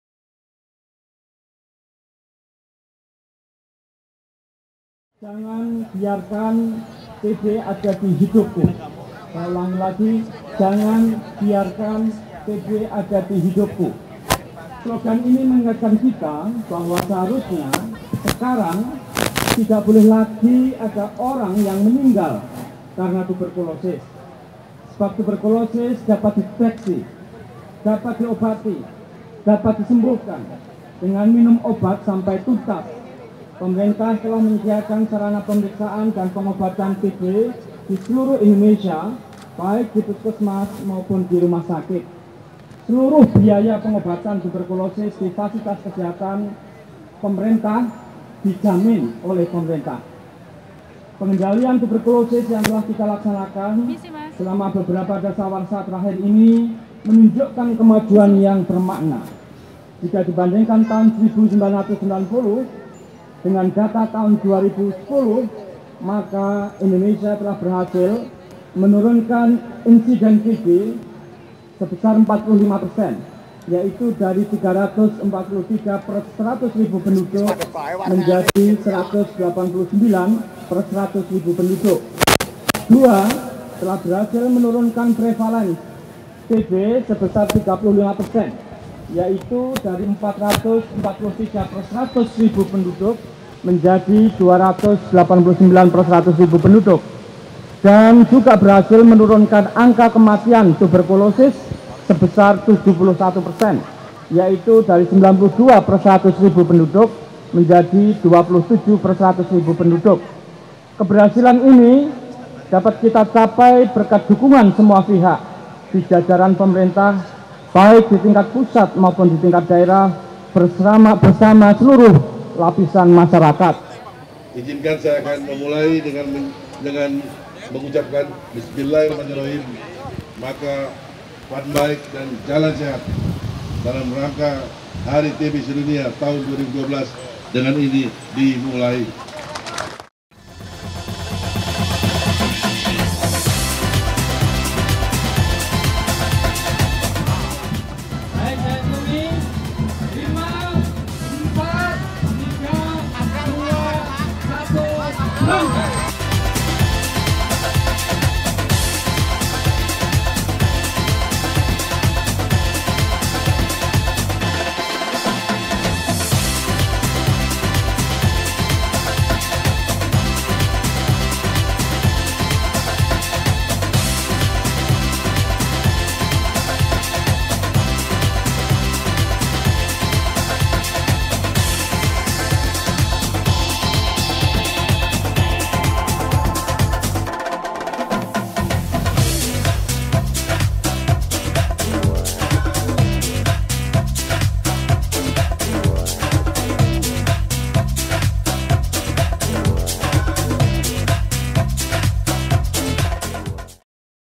Jalan Sehat dan Fun Bike dalam Rangka Hari TB Sedunia 2012
Acara Jalan Sehat dan Fun Bike di selenggarakan dalam Rangka Hari TB Sedunia, pada tanggal 01 April 2012 di Monas, Jakarta